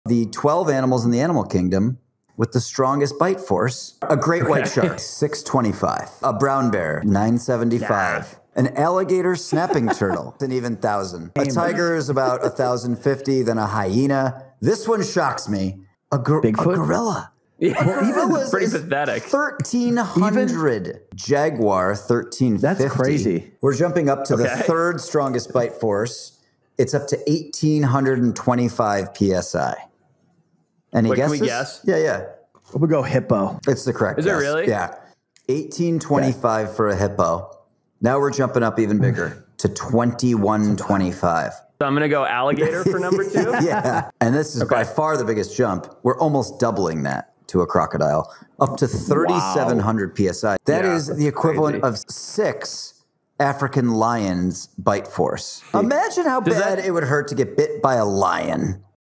The 12 Strongest Bite Forces in The Animal Kingdom
Forrest Galante & The Wild Times crew discuss the 12 strongest bite forces in the animal kingdom. Some of these might surprise you.